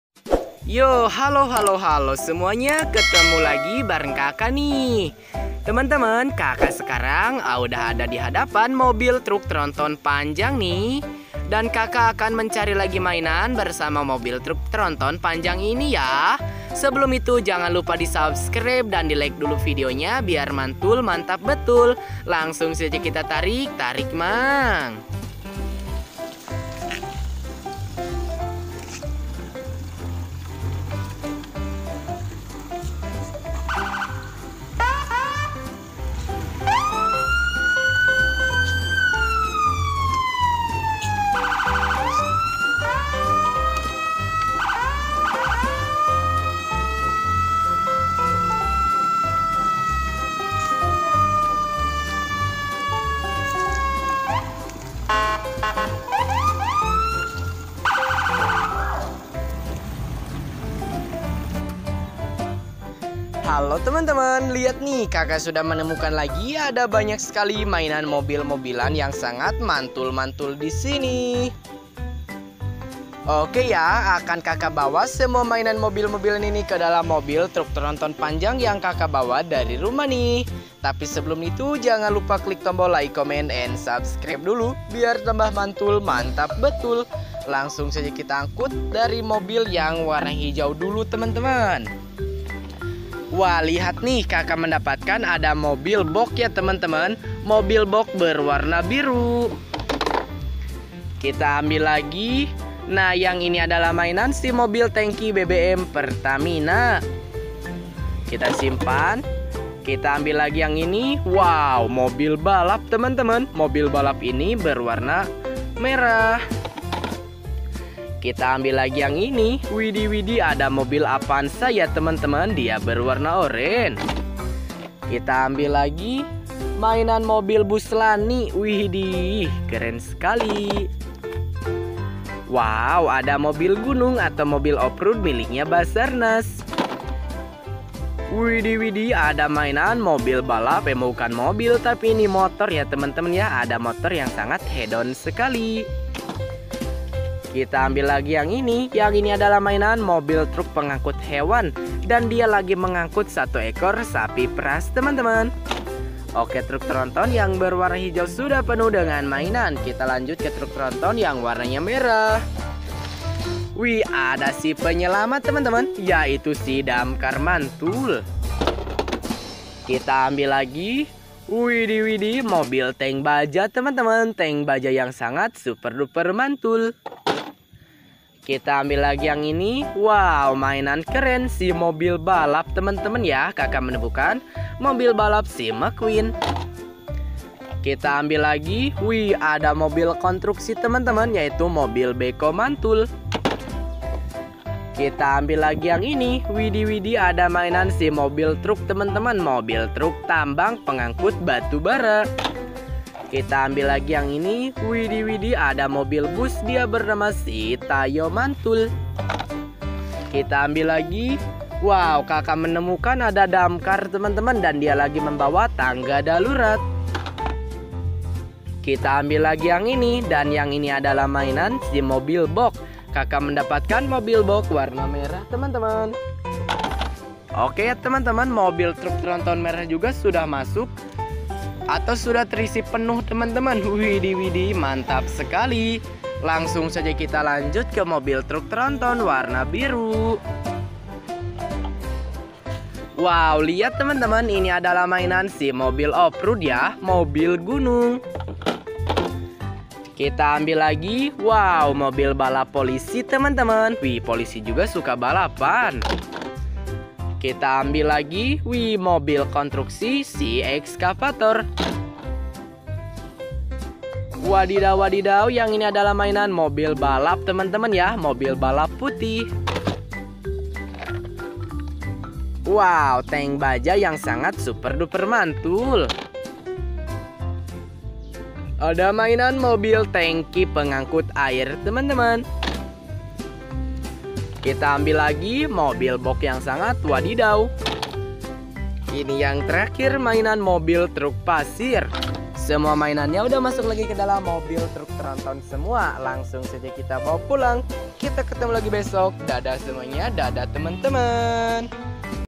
Mencari Mainan dengan Truk Angkut Episode Terbaru part13
Bermain mencari dan menemukan mainan. Banyak sekali mainan yang diangkut dengan truk